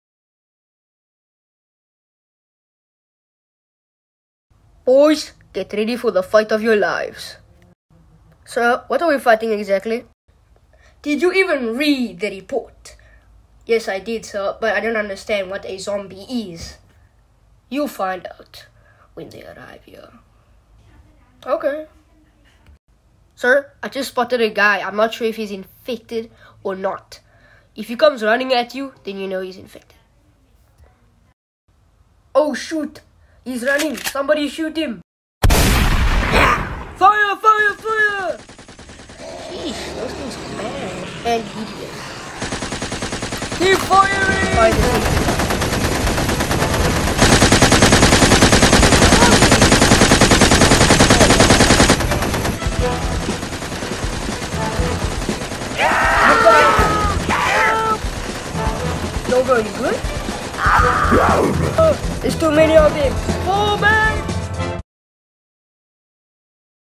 Lego Zombie Attack remake (part 1)
A Lego stop motion about the zombie attack in Lego city.